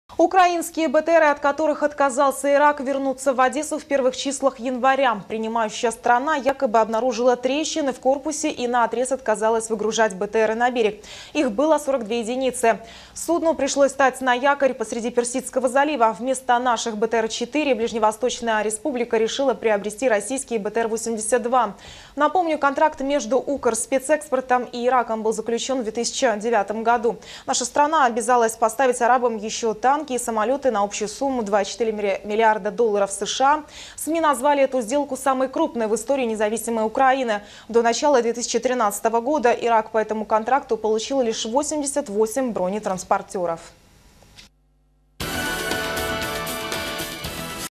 Забракованные иракцами БТРы скоро будут в Одессе
Украинские БТРы, от которых отказался Ирак, вернутся в Одессу в первых числах января. Принимающая сторона якобы обнаружила трещины в корпусе и наотрез отказалась выгружать БТРы на берег.